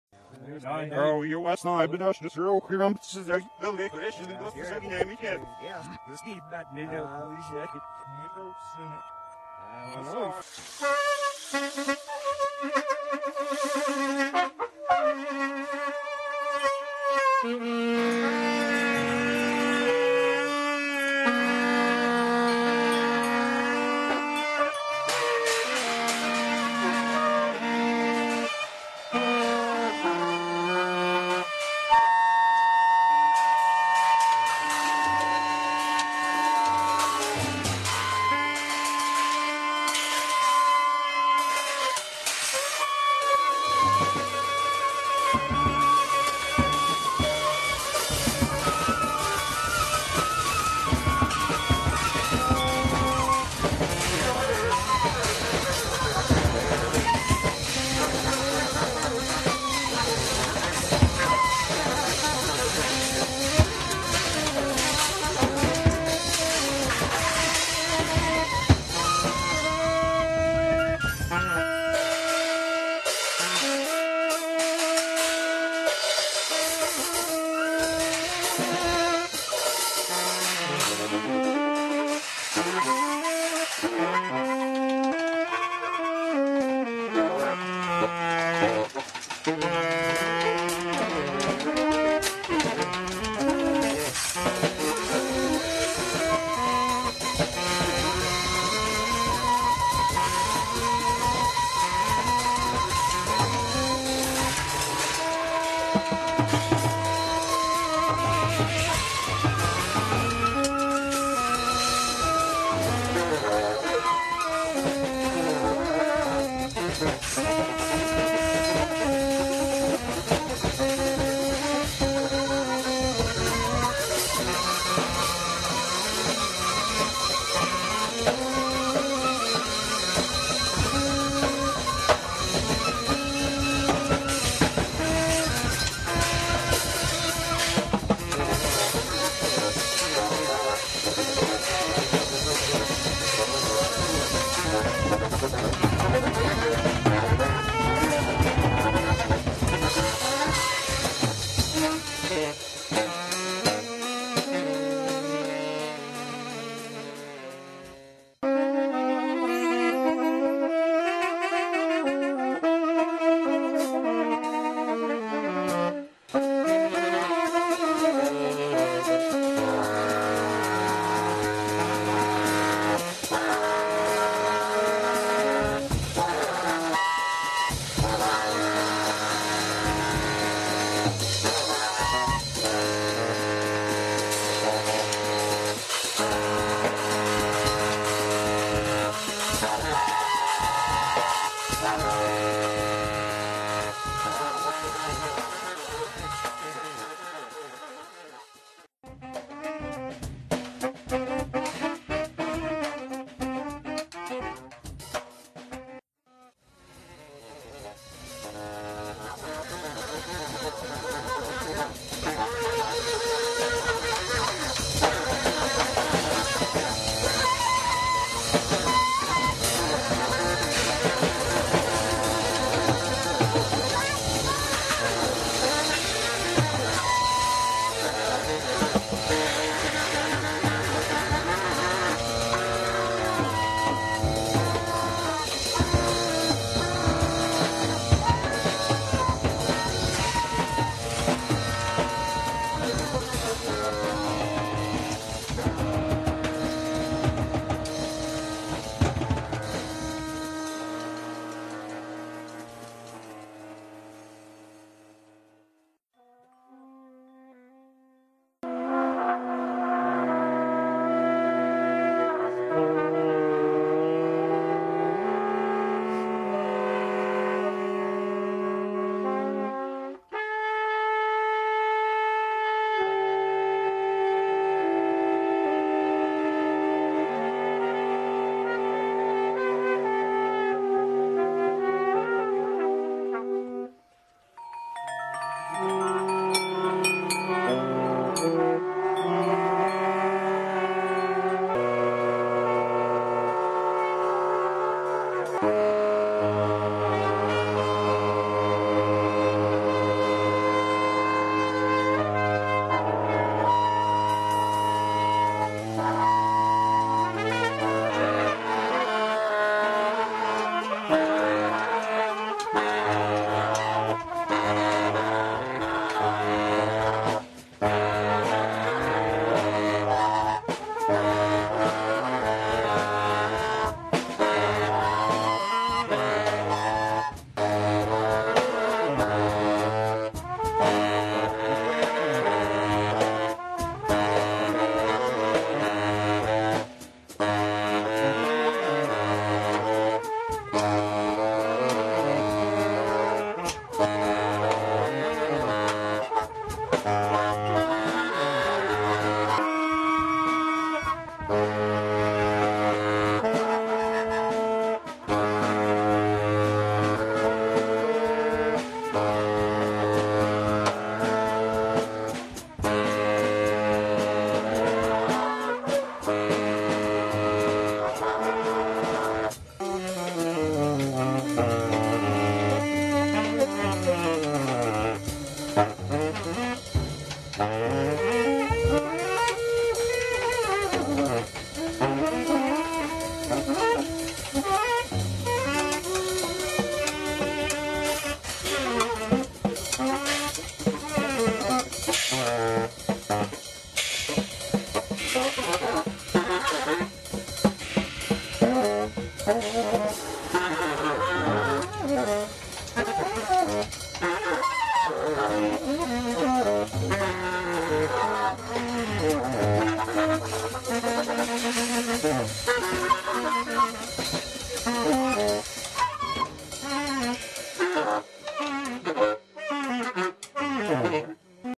Free Jazz Introduction
The Organic Garden
Introduction 9-20-11
Jamie : tenor sax, chimes
Steve : tenor sax, chimes
Nick : drums, french horn, trumpet, chimes